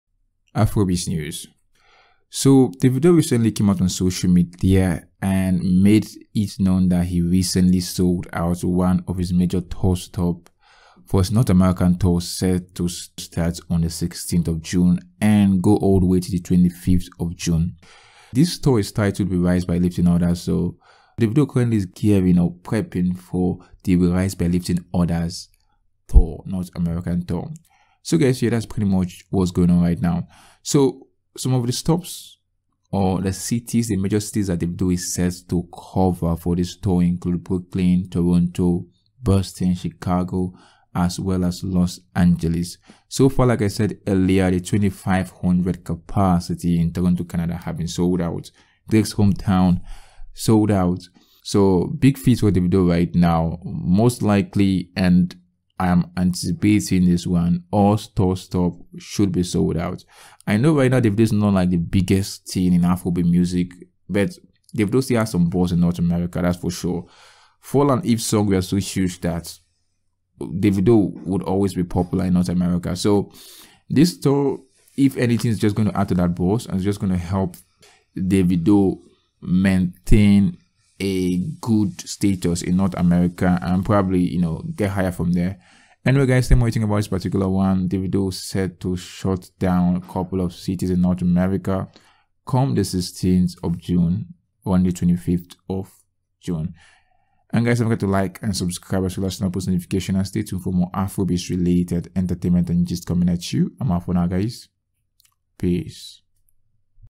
Davido Just SOLD Out Toronto Canada
Award-winning singer and performer David Adeleke, who is popularly known as Davido, took to his social media platform to officially announce selling out all tickets at the Toronto, Canada, venue for his North American Tour-themed 'We Rise By Lifting Others.'